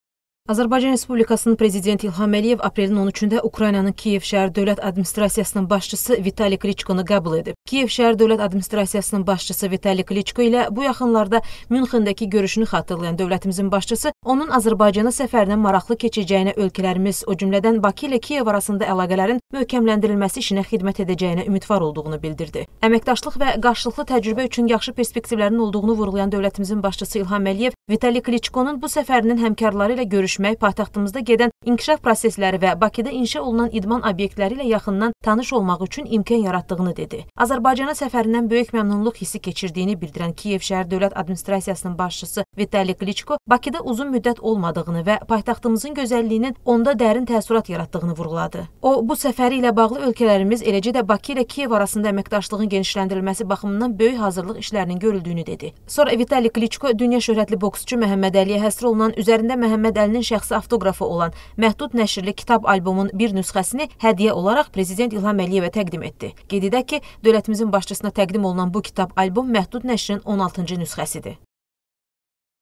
Azərbaycan Prezidenti Kiyev Şəhər Dövlət Administrasiyasının başçısını qəbul edib
Azərbaycan Prezidenti İlham Əliyev Kiyev Şəhər Dövlət Administrasiyasının başçısını qəbul edib